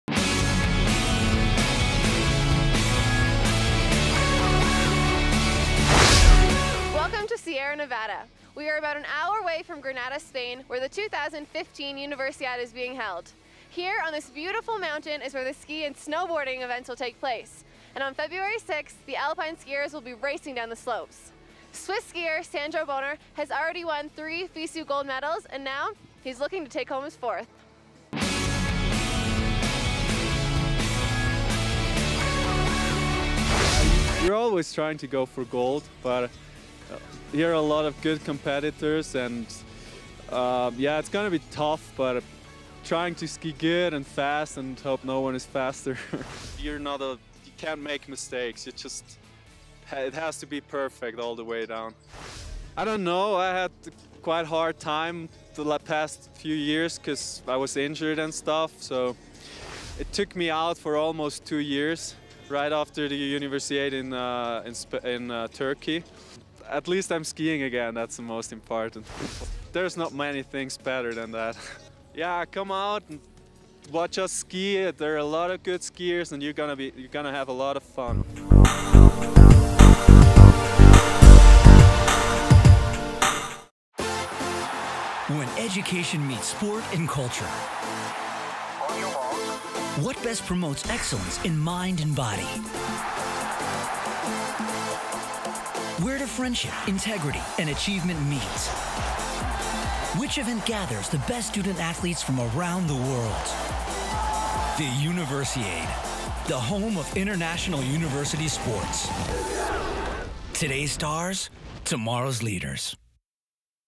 Sandro Boner is a Swiss alpine ski racer - 27th Winter Universiade, Granada, Spain
Sandro Boner is a Swiss alpine ski racer, who won three gold medals at the 2009 Winter Universiade in Erzurum, Turkey.

Almost immediately after the Games, Boner suffered a serious injury that took him out of competition for over two years.

Now, the University of Distance Learning, Pfaffikon student, is in Granada at the 2015 Winter Universiade and he is ready to bring home his fourth FISU gold medal and prove to himself that he is still a champion.